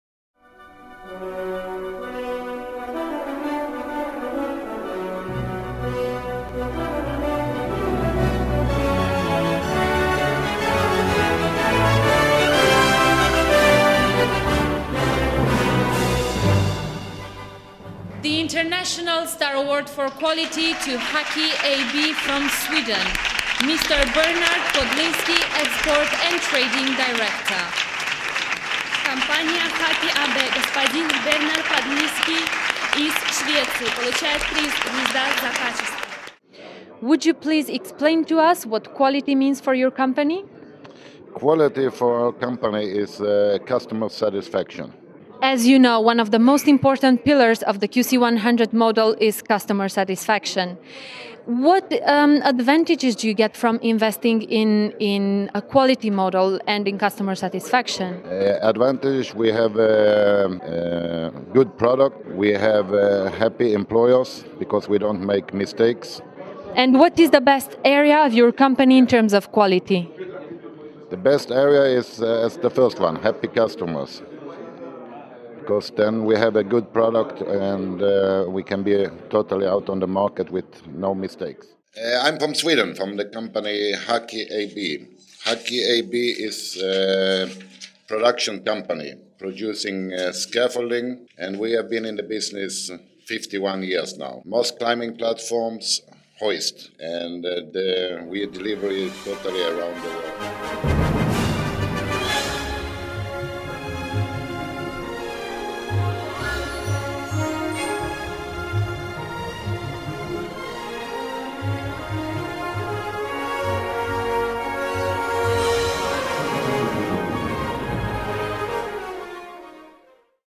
HAKI AB
HAKI AB from Sweden was awarded with BID's International Quality Award in Geneva.